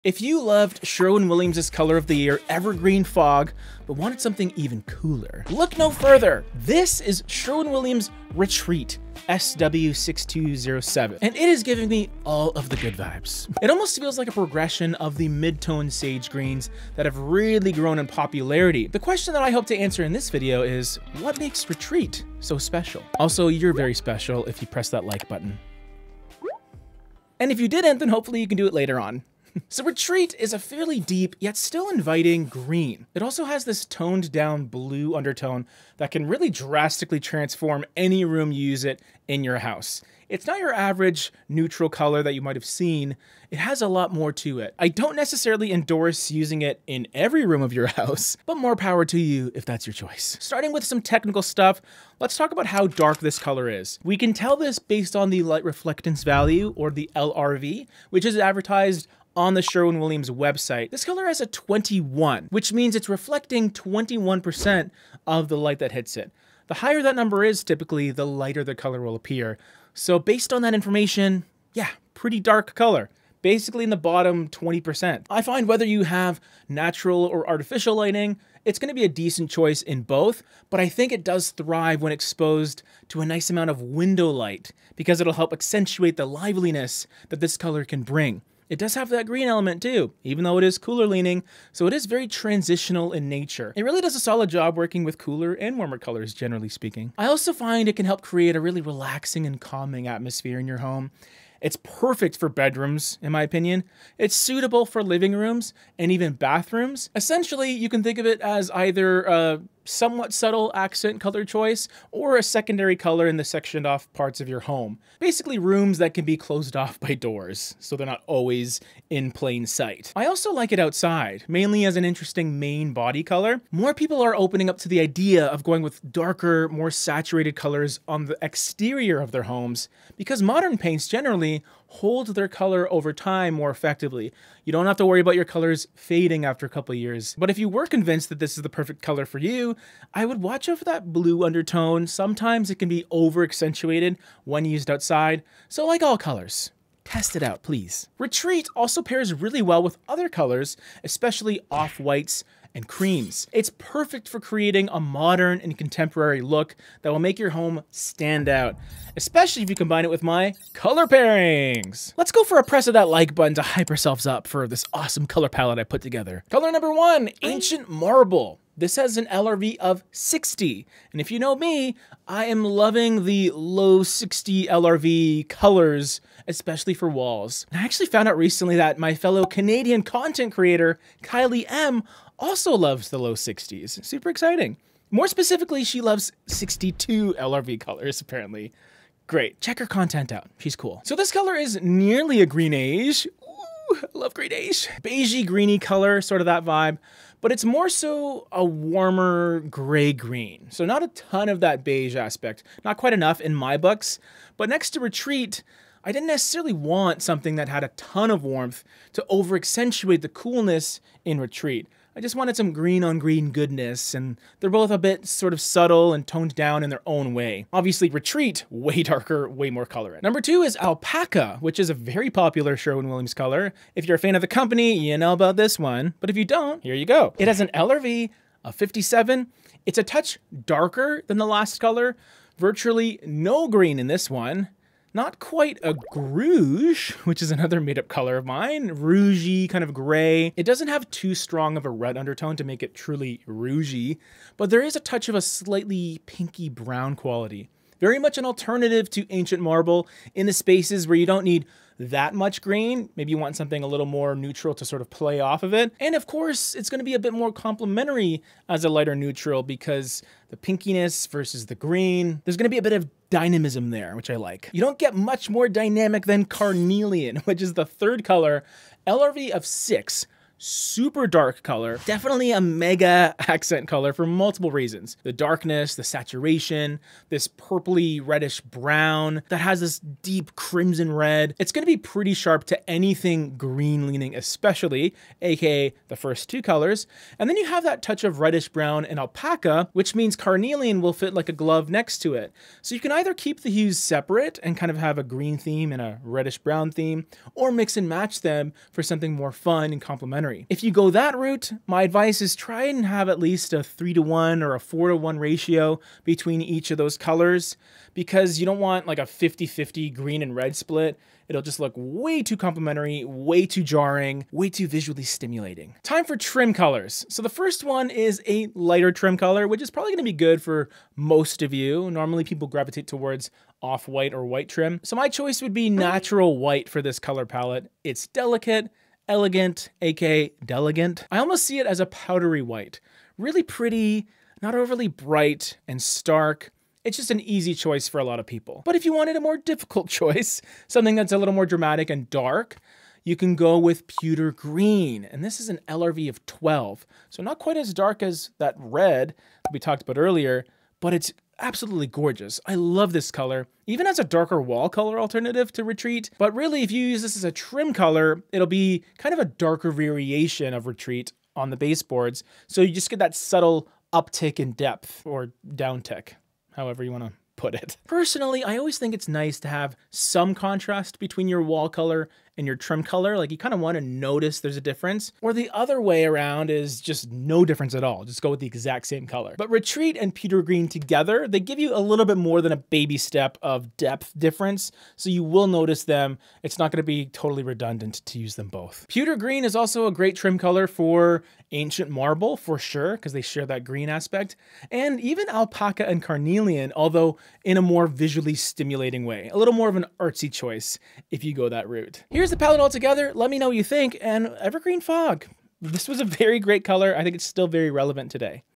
WOW! You Won't Believe This Trendy Sherwin Williams Color!
In this video, we'll show you a trending Sherwin Williams green color that you'll never believe! This astonishing color is sure to turn your home into a masterpiece!

If you're looking for a trendy green color that will make your home stand out, then you need to check out Sherwin Williams Retreat! This color is sure to turn your home into a stunning gem!